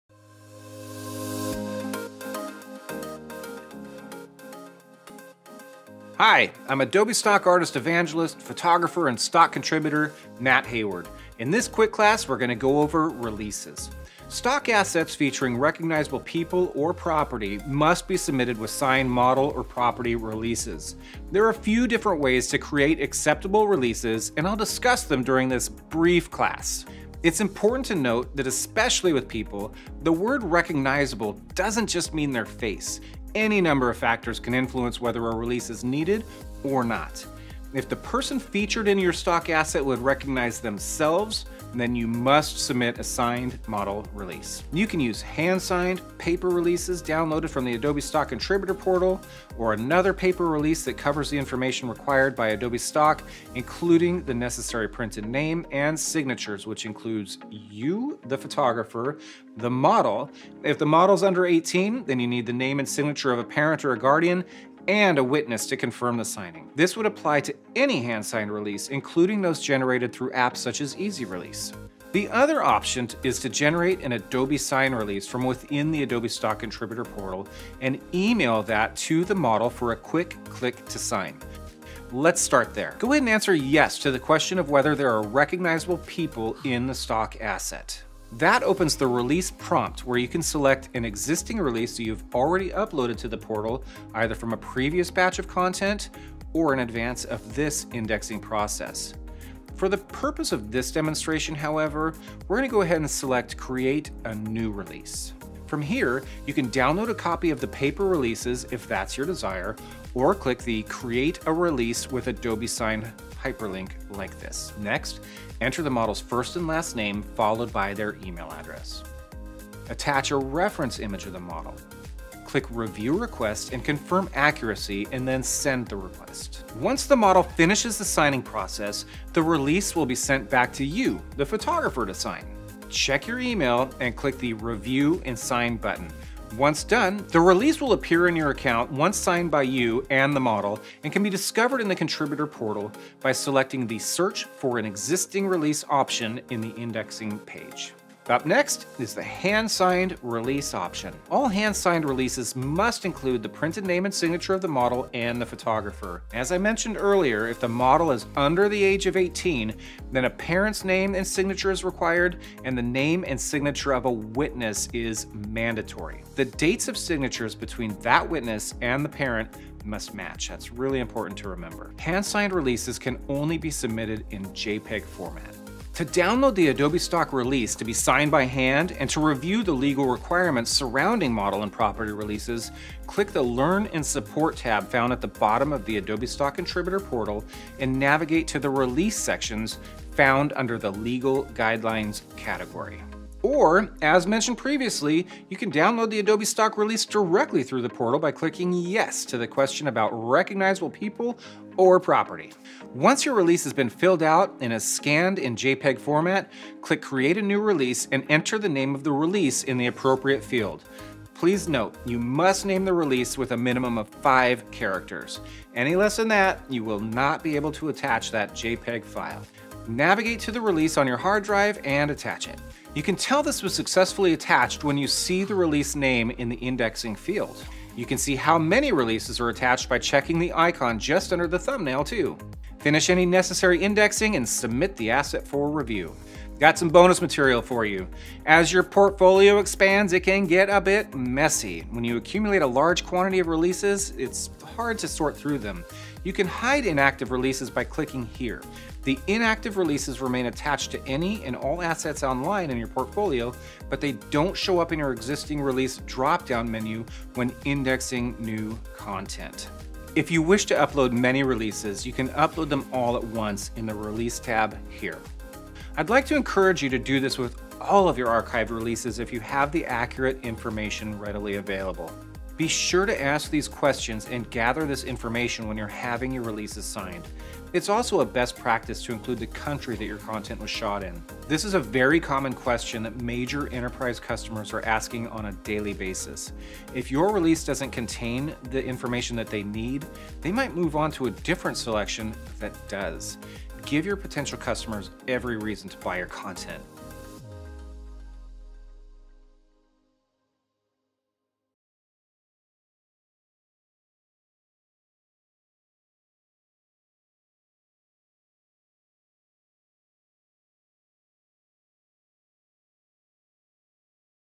Quick Class 6 of 10 | Create and Submit Model/Property Releases in the Portal | Adobe Creative Cloud
When your photos, videos, and other files include recognizable people or property, you’re required to obtain the models’ or owners’ permission before uploading your files to Adobe Stock. In this Quick Class, Adobe Stock Evangelist Mat Hayward explains how to create and upload model and property releases in the Adobe Stock Contributor portal.

Chapters:
00:00:14 - Key information about releases
00:01:12 - Generating a release
00:02:28 - Hand-signed release options
00:03:23 - Final steps and inactive releases